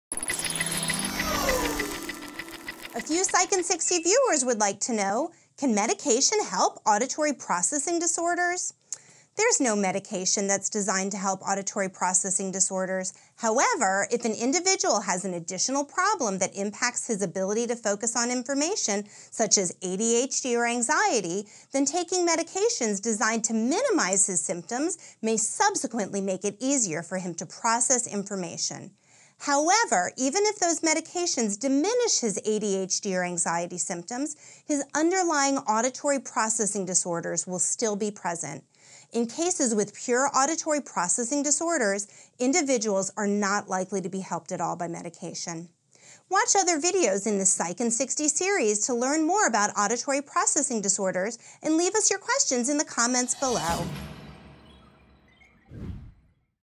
Can Medication Help Auditory Processing Disorders? | Psych in 60
There is no medication that is designed to help Auditory Processing Disorders.

If an individual has an additional problem that impacts his/her ability to focus on information, such as ADHD or anxiety, then taking medications designed to minimize his/her symptoms may subsequently make it easier for him/her to process information.

However, even if those medications diminish his/her ADHD or anxiety symptoms, his/her underlying auditory processing disorders will still be present.

In cases with pure Auditory Processing Disorders, individuals are not likely to be helped at all by taking medication.